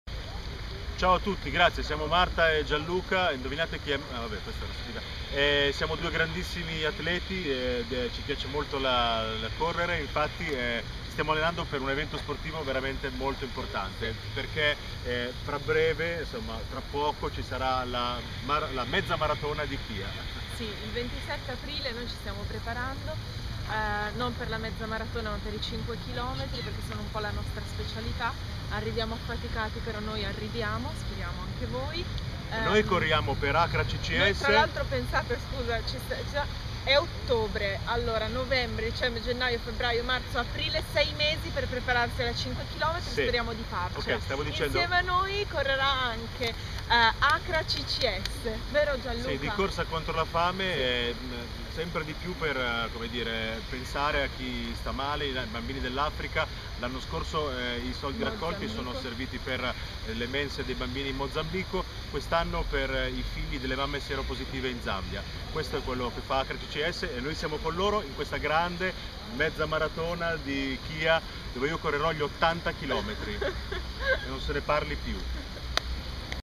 Il running secondo Marta & Gianluca
Marta Zoboli e Gianluca De Angelis, in arte Marta & Gianluca, il duo comico dell'area Zelig celebre per le esilaranti battute che si lanciano durante i loro incontri sul palco, hanno deciso di provarci. Parteciperanno per Acra-Ccs alla prossima Chia Laguna Half Marathon (21K, 10K e 5K) in programma il 27 aprile 2014.